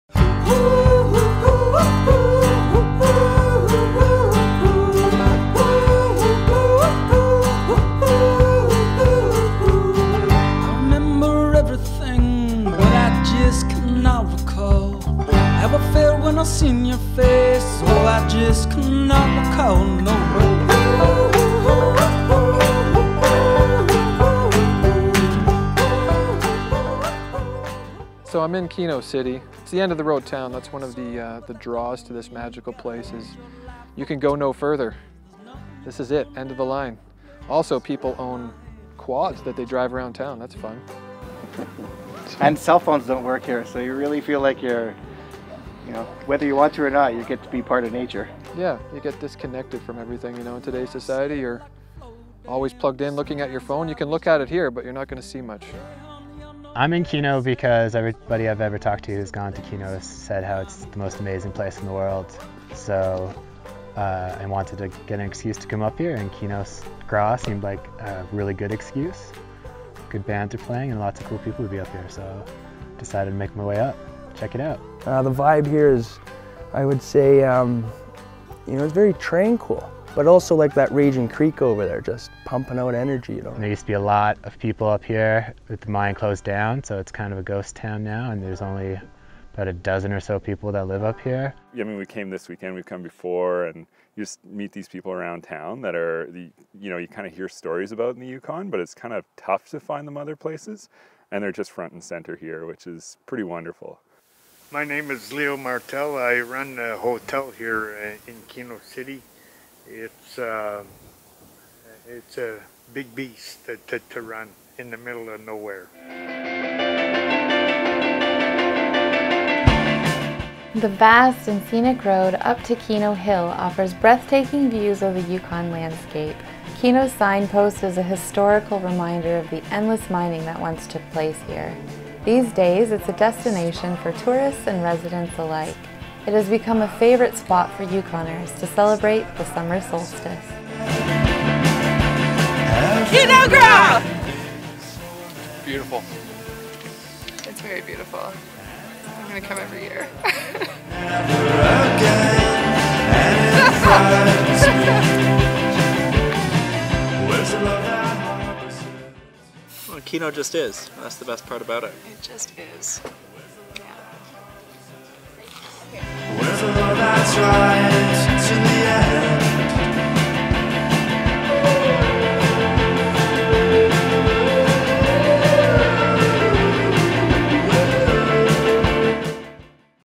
Keno City, Yukon - a town at the end of the road
Keno City, Yukon is home to colorful characters, stunning scenery and living history. Once a booming frontier mining town, Keno City was named after the popular gambling game. Today, this sleepy hamlet is a rustic cluster of buildings and home to an eclectic mix of artists, miners and old timers with fewer than 20 full-time residents.